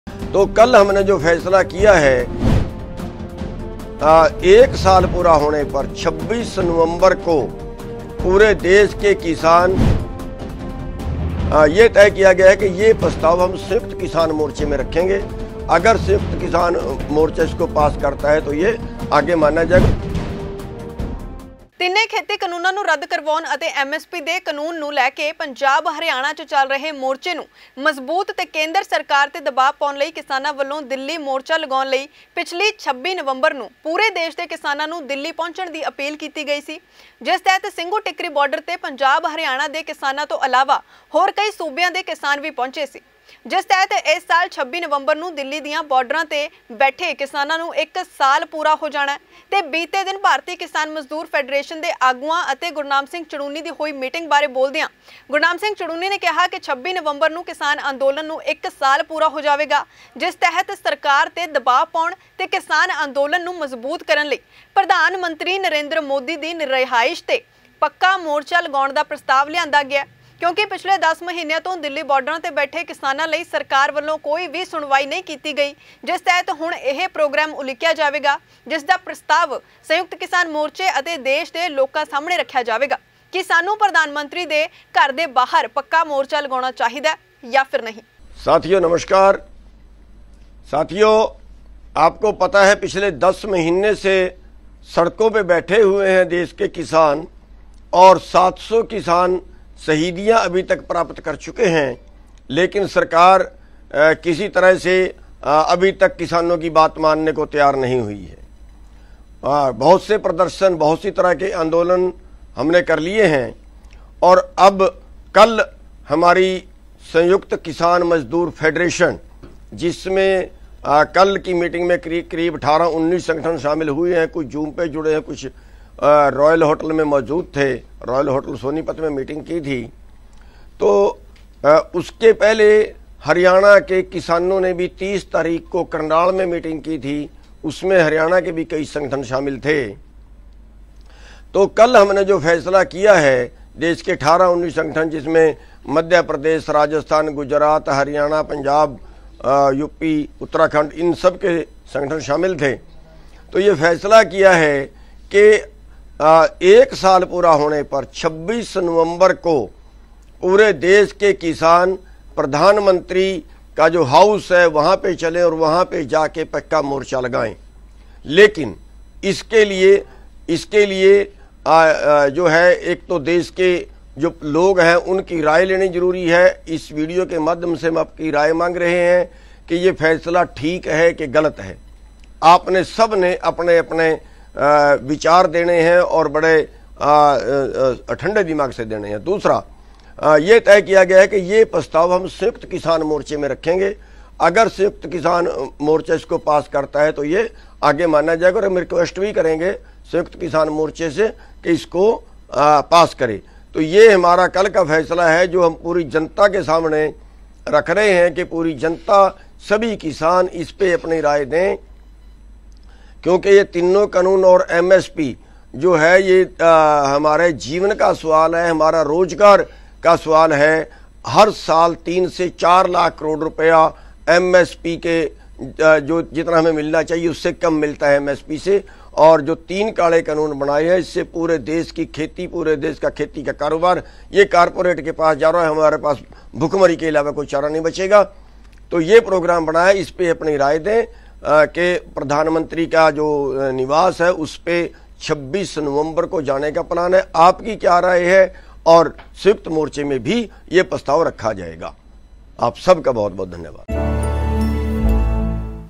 'Andolan ਨੂੰ 1 ਸਾਲ ਪੂਰਾ ਹੋਣ ਤੇ PM House ਤੇ ਲੱਗੇ ਪੱਕਾ Morcha'
ਕਿਸਾਨ ਅੰਦੋਲਨ ਨੂੰ ਲੈਕੇ ਗੁਰਨਾਮ ਚੜੂਨੀ ਵੱਲੋਂ ਸੰਯੁਕਤ ਮੋਰਚੇ ਨੂੰ ਪ੍ਰਸਤਾਵ
'ਅੰਦੋਲਨ ਨੂੰ 1 ਸਾਲ ਪੂਰਾ ਹੋਣ ਤੇ PM ਦੀ ਰਿਹਾਇਸ ਤੇ ਲੱਗੇ ਪੱਕਾ ਮੋਰਚਾ'

Vision Punjab Tv, Gurnam Singh Charuni, Kisan Andolan, Sanyukat Morcha,
PM House, Kisan Morcha